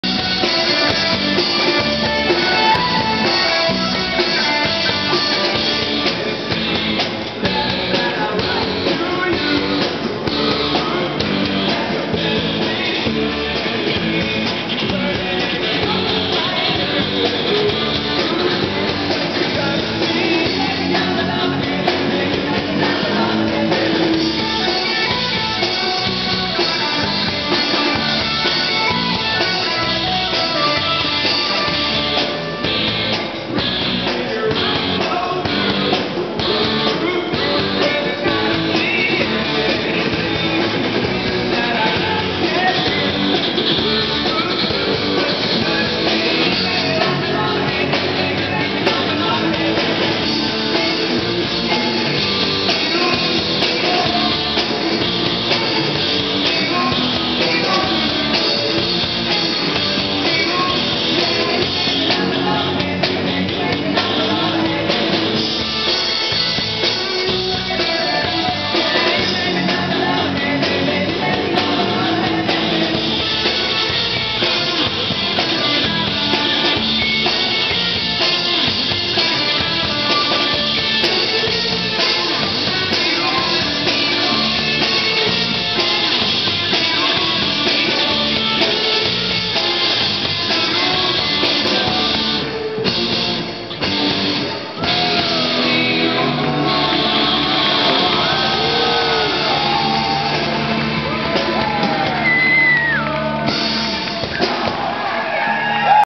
Virgin Mary (VM) @ Aloha Tower on March 31, 2012
Virgin Mary (VM) performing at Aloha Tower on March 31, 2012